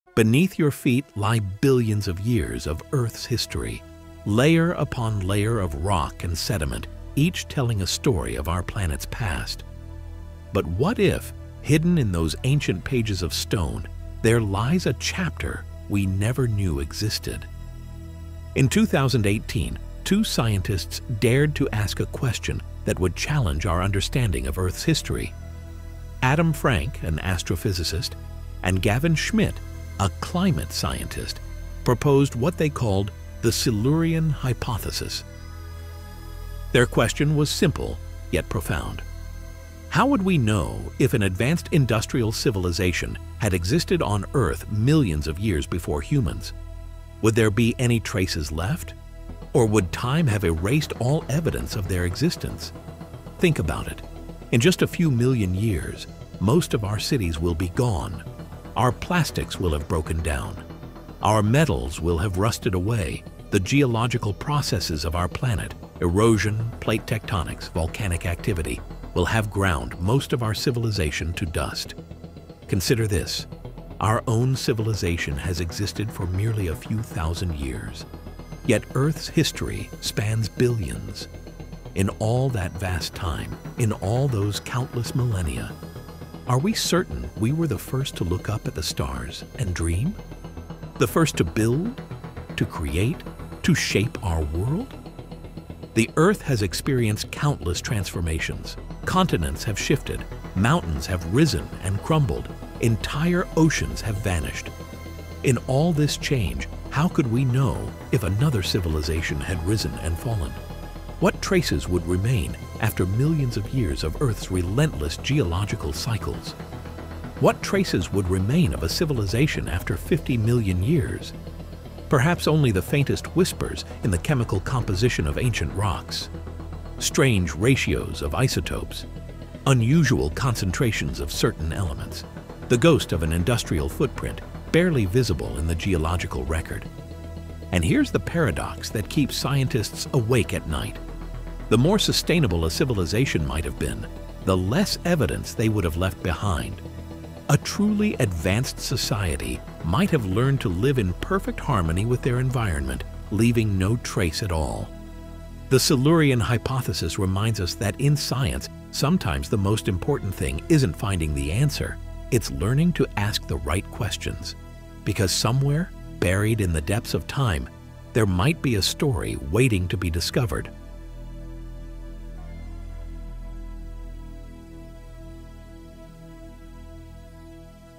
The Silurian Hypothesis: What If Humans Weren't First?
What if we weren't the first advanced civilization on Earth? Two scientists propose a mind-bending question: Could an industrial civilization have existed millions of years before humans... and would we even be able to tell? Explore the fascinating Silurian Hypothesis, where astrophysicist Adam Frank and climate scientist Gavin Schmidt investigate how we might detect ancient civilizations in Earth's geological record. From vanished cities to chemical signatures in ancient rocks, discover why finding evidence of such a civilization might be nearly impossible... even if it did exist.